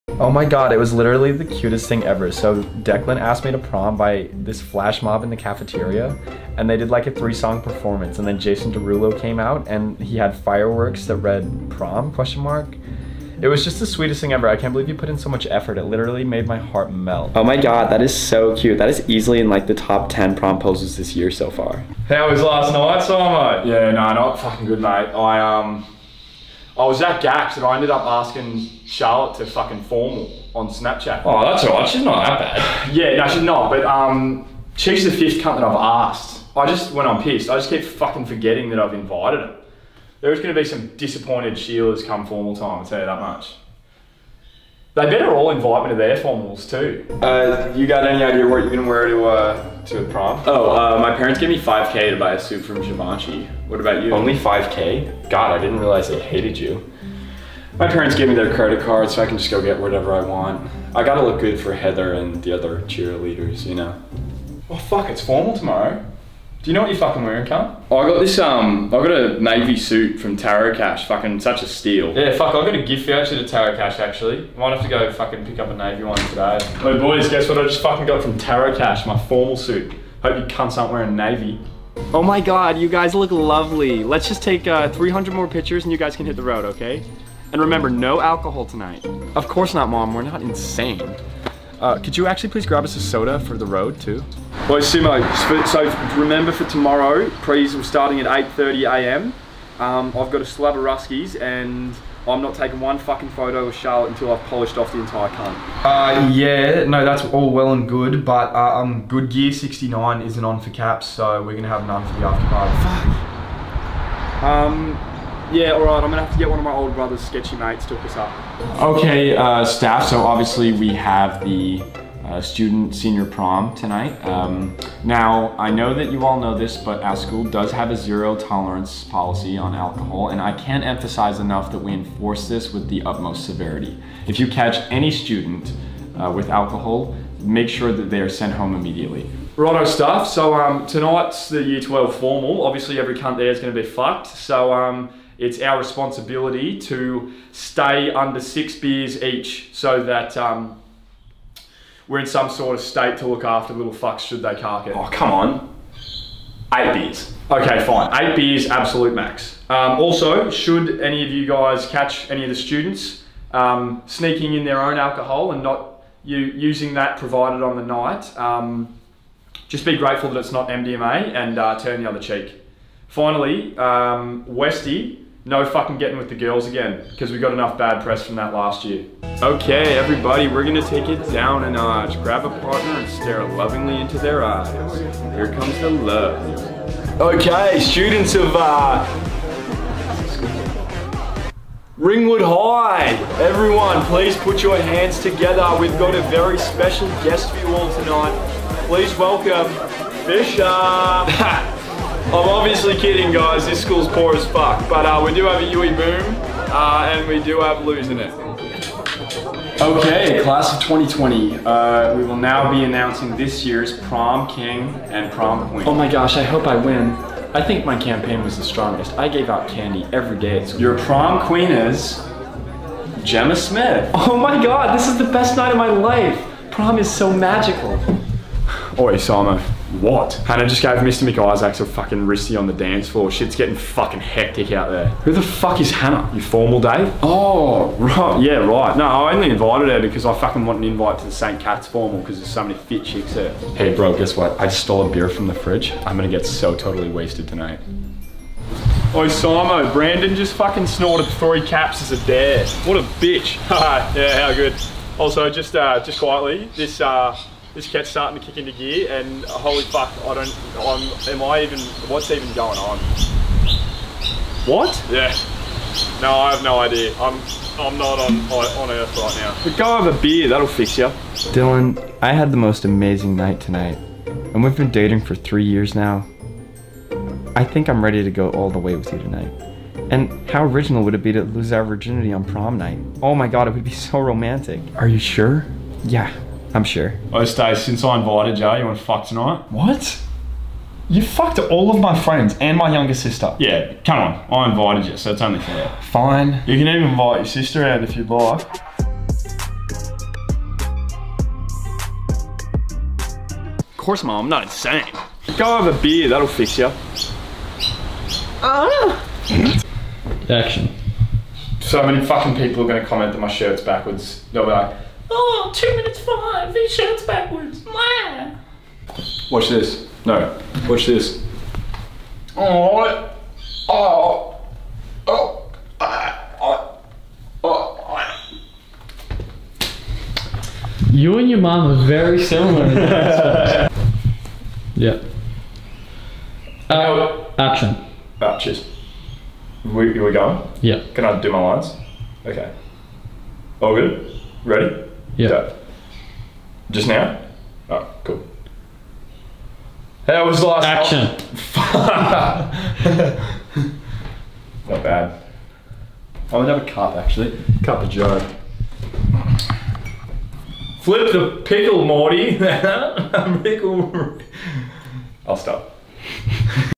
American Prom vs Australian School Formal
This is exactly how my school formal went.